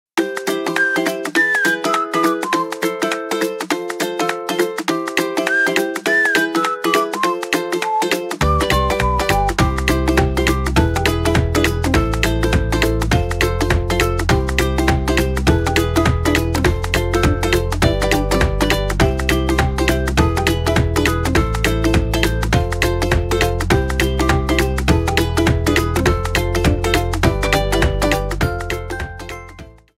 Colourful Painting with edding Colour Happy Big Box for Valentine's Day
Unleash your creativity and add some colour to your life with this painting tutorial! Using the edding Colour Happy Big Box, we show you how to create multiple colourful hearts using our painting template. Perfect for a DIY home decor project or as a gift for a loved one. Watch the video now and start drawing!